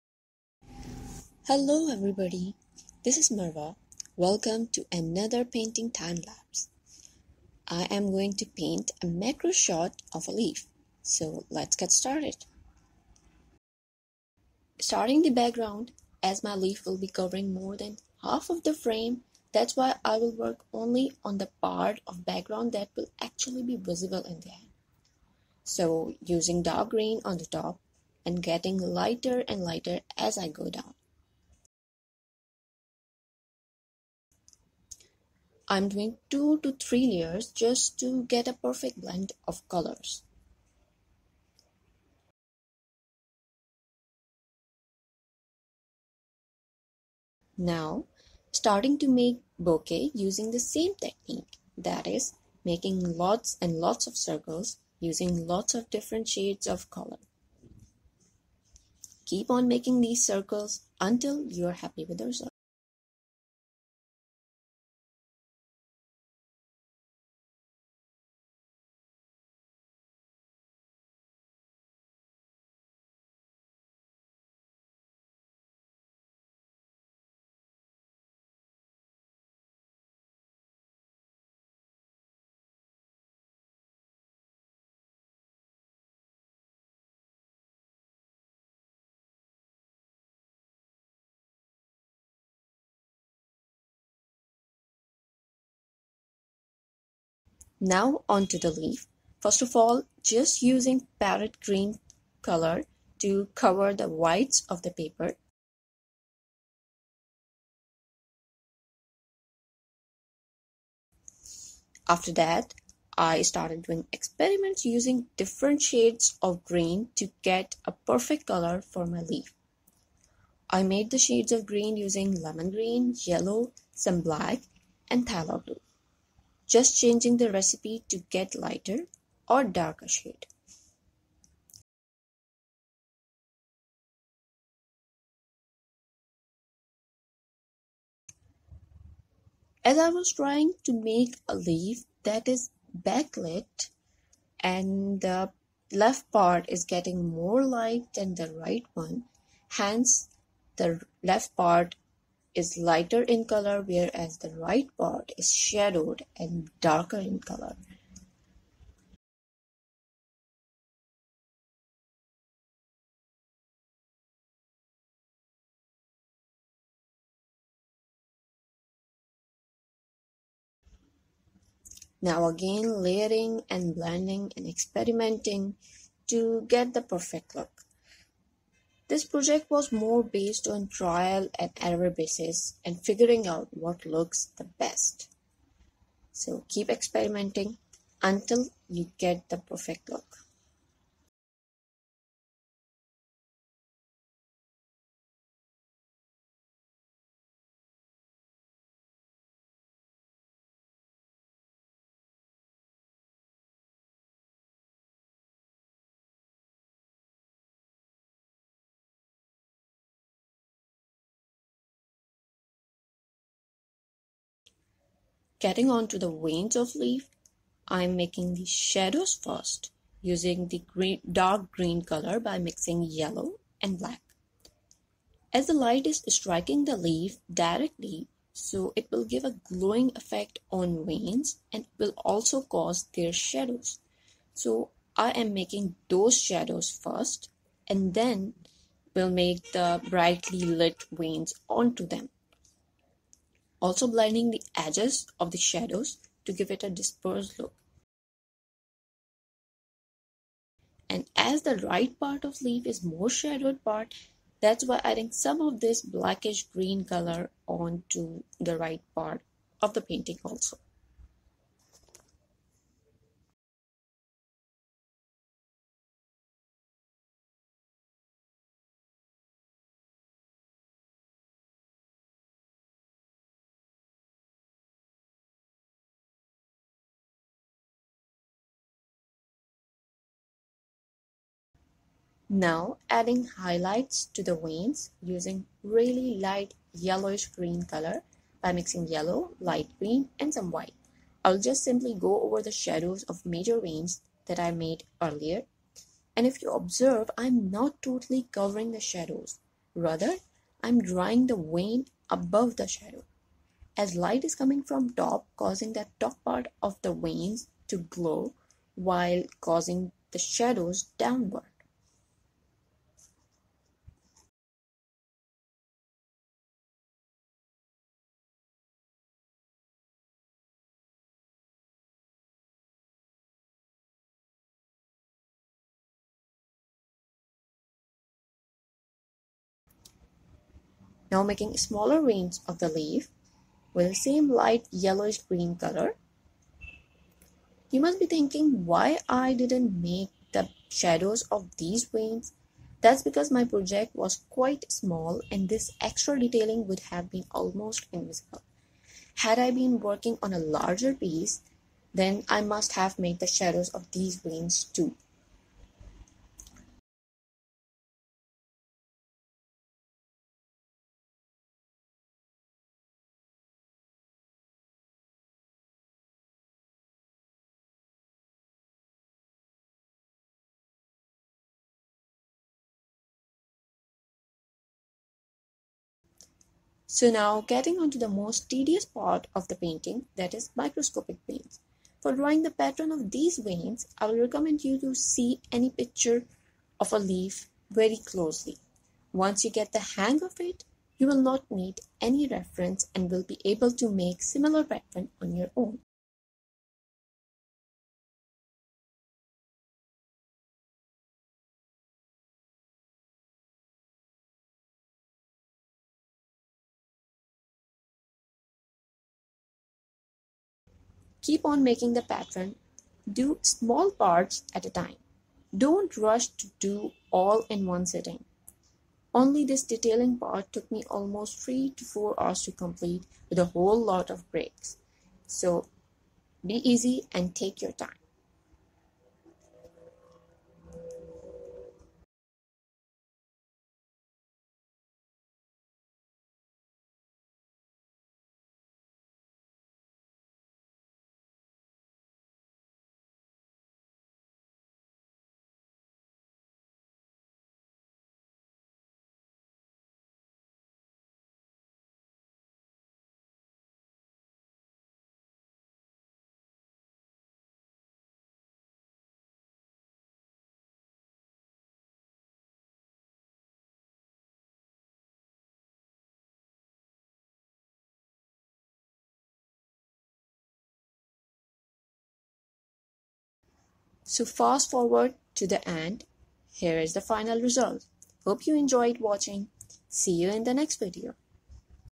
Paint a MACRO Leaf in Acrylics | Painting Timelapse
Here is a painting timelapse for macro photograph of a leaf.